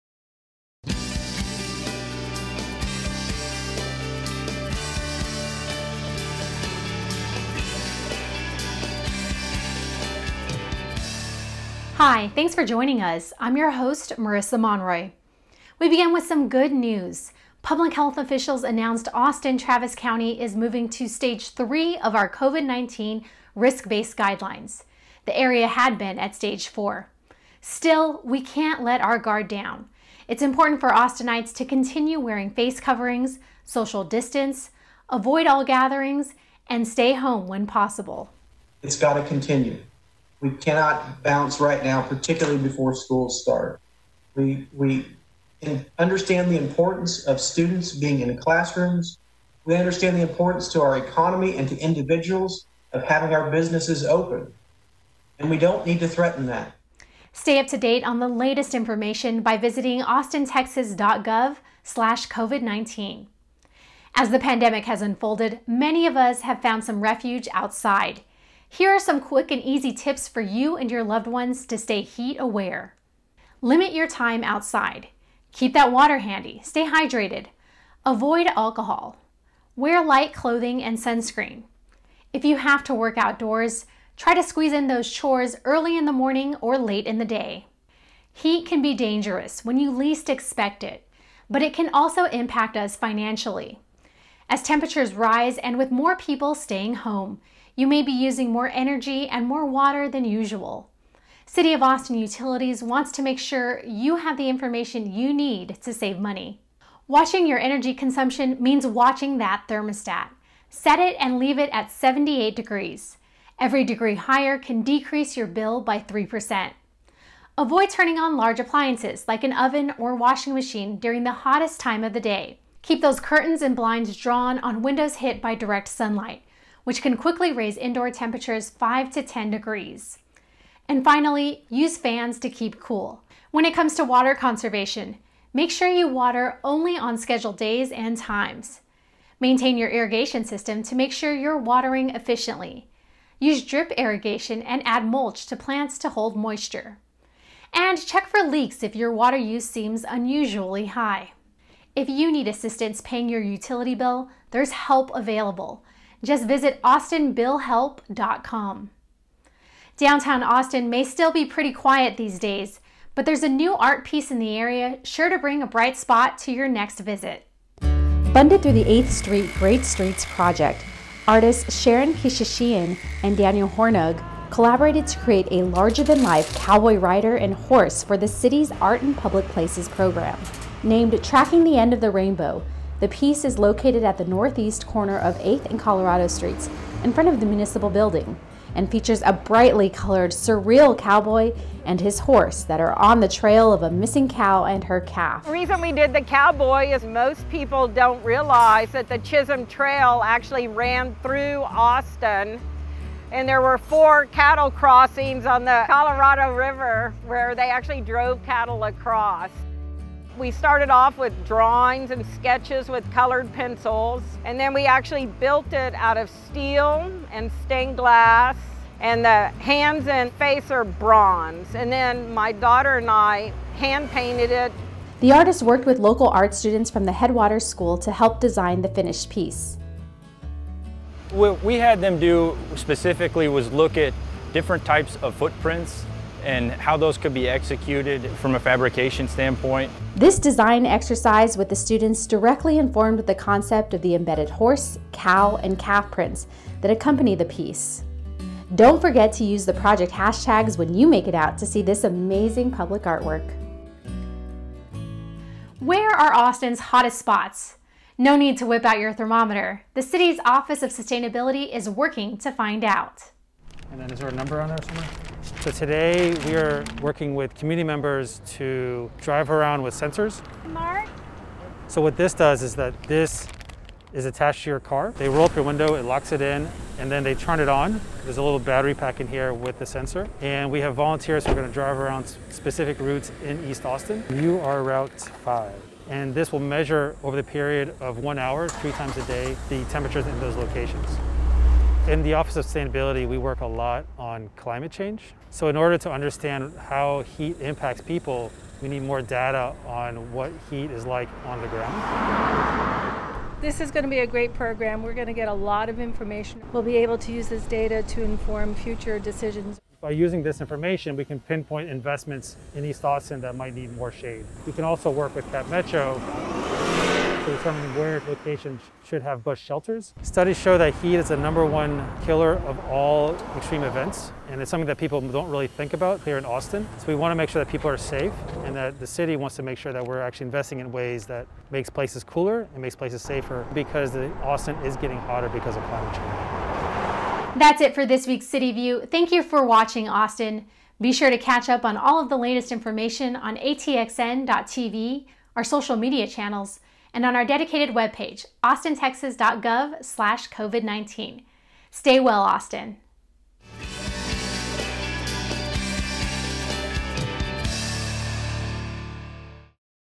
CityView @ Home Episode 18
This episode of CityView @ Home contains the following stories: Austin shifts to stage 3 in COVID-19 risk-based guidelines. We go over some heat awareness and summer savings tips. There is a new public art piece in Downtown Austin, and we look into the City of Austin's heat mapping project.
To see more episodes from our CityView at Home series,